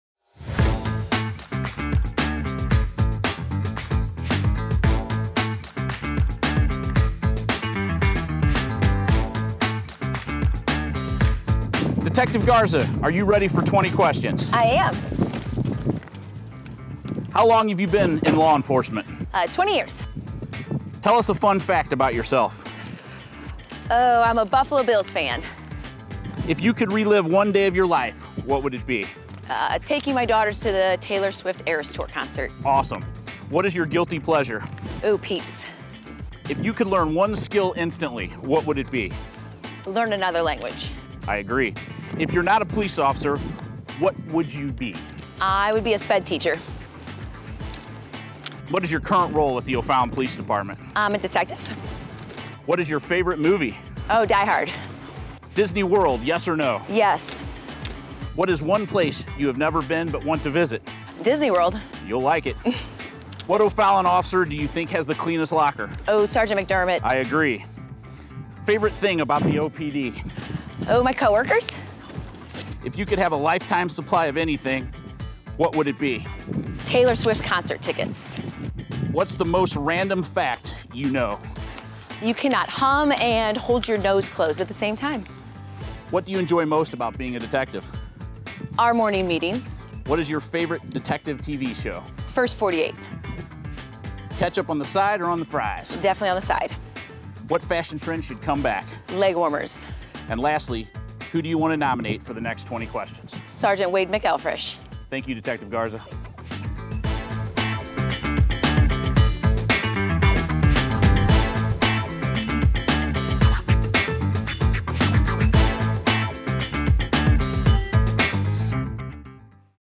20 Questions with Det. Holly Garza | O'Fallon, Missouri
Get to know your O'Fallon Police Department officers! Our latest installment of 20 Questions is with Detective Holly Garza.